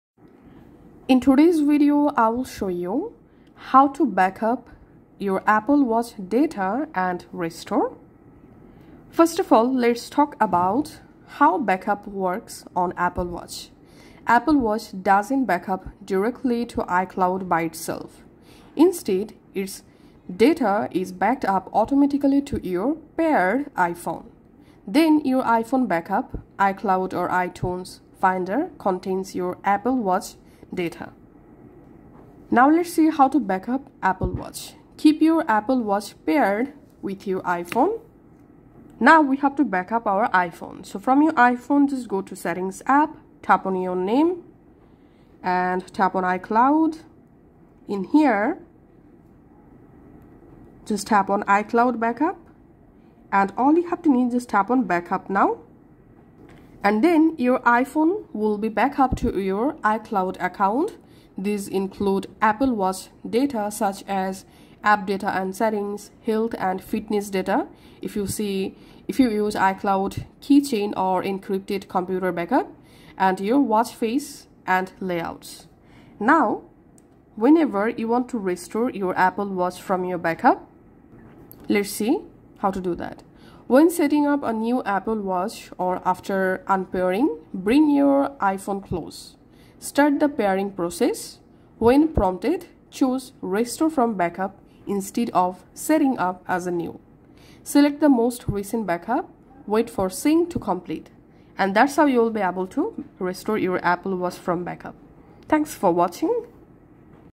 How to Backup Apple Watch Data & Restore — Easy Guide
Learn how to backup and restore Apple Watch data ⌚💾. This step-by-step guide shows you how to safeguard your apps, settings, and health data and restore it easily whenever needed.